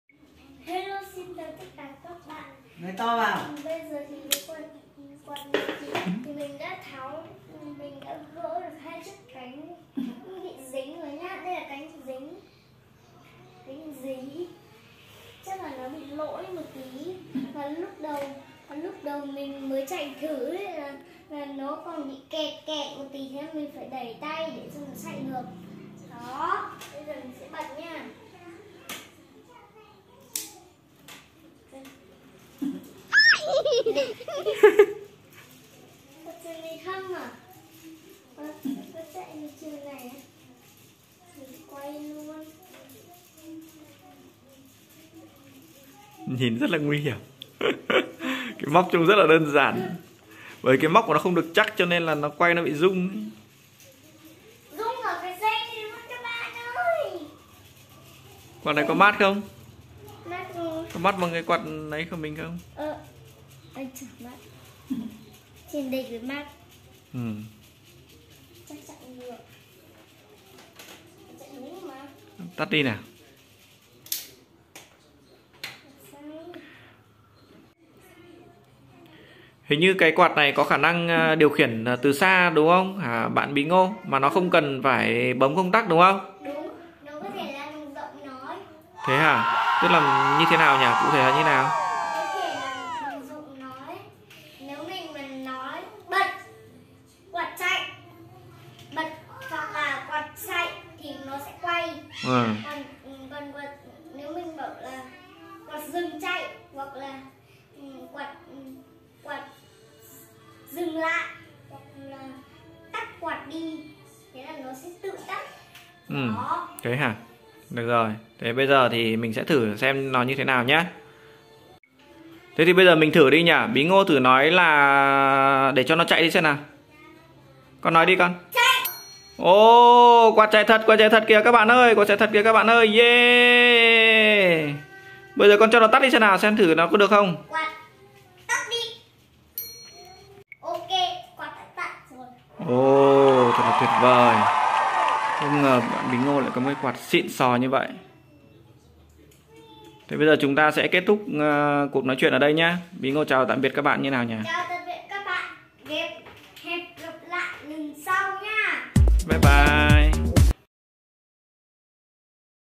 Quạt trần siêu cấp của bạn Bingo p2 (hấp dẫn với quạt tự động) 😁
Quạt siêu cấp điều khiển bằng giọng nói của Bingo 🤣🤣🤣🤣